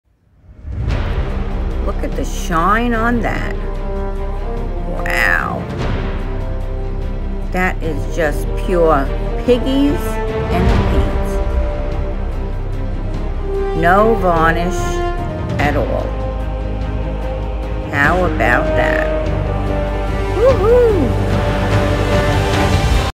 Sneak Peek What does a cheesecake pan have to do with a sheleeart bloom?
TLP and paints only NO SILICONE @olgasoby inspired
Creation video tomorrow at 4:30 Pacific Time link will follow...
Come to tomorrow's Friday with friends where I will show you how I was inspired to use a technique from Olga Soby and make it my own...today is just the first of my series where I explore all the different ways this technique can be used with the bloom technique...don't miss this you'll be excited when you see how much fun you can have with this!